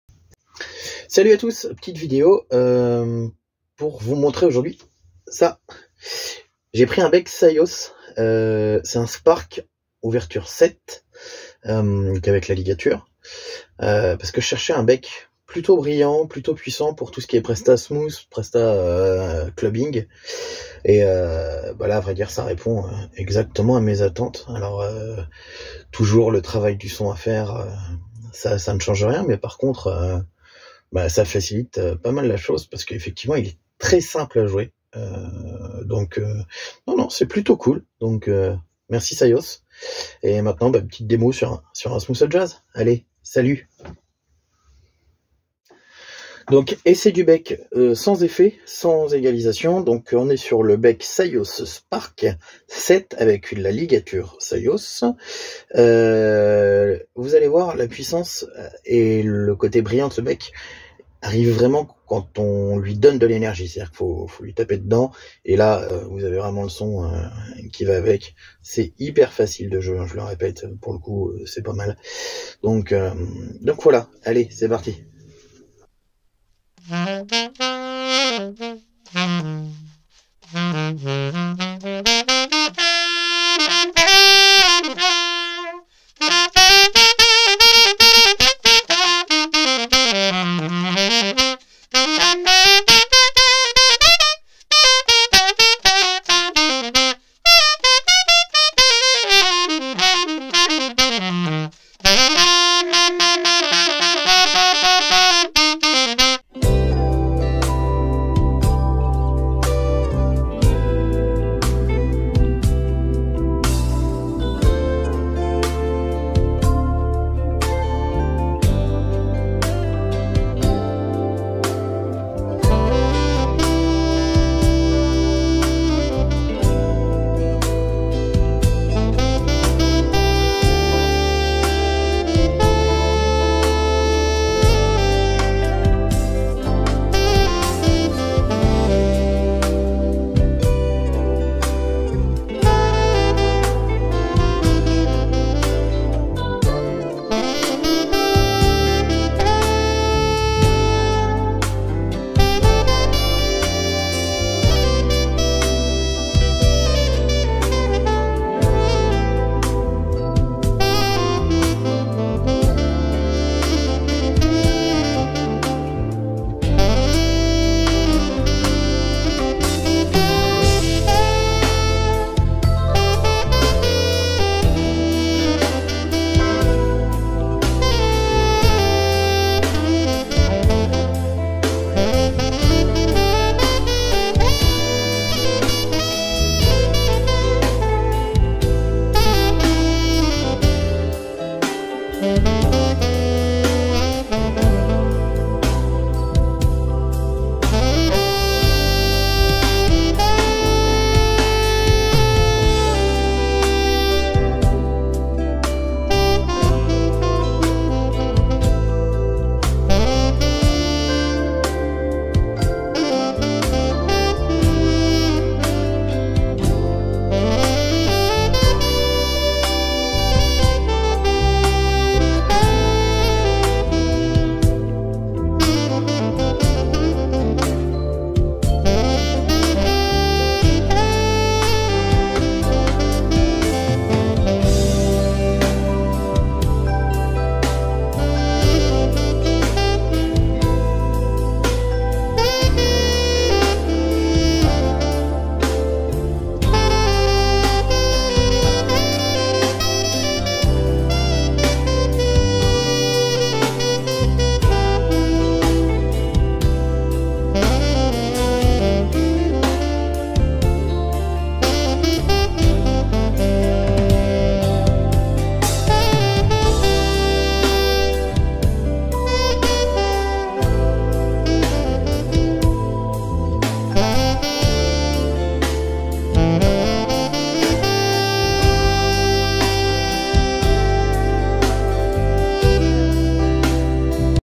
test bec @Syosmusic alto spark 7.
voilà un premier test au déballage du bec syos spark ouverture 7 pour alto.
il faut que je trouve le couplé anche bec idéal les 3 de syos (légèrement retouchées sont pas mal du tout) la c'est une legere american cut